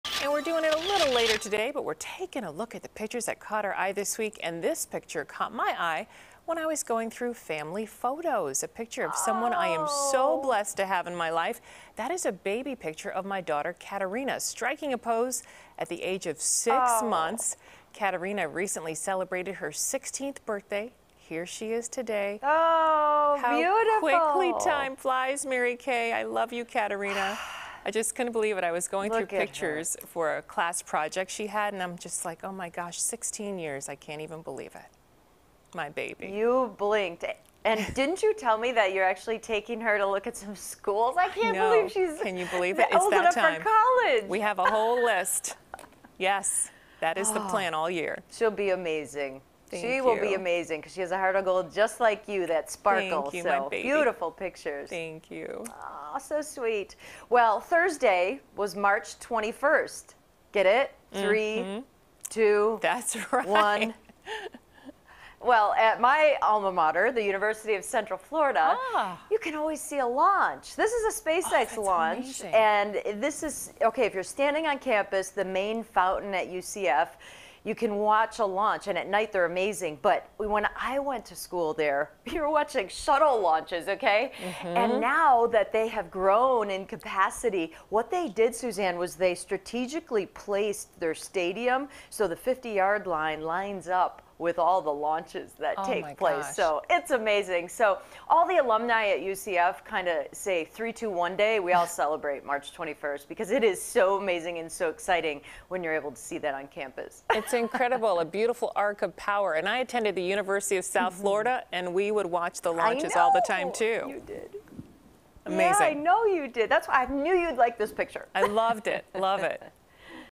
Family photo shows how time flies, SpaceX rocket launch near UCF campus
This week, Suzanne Le Mignot shares a photo that shows how time flies, While Mary Kay Kleist shares a SpaceX rocket launch near her alma mater, the University of Central Florida.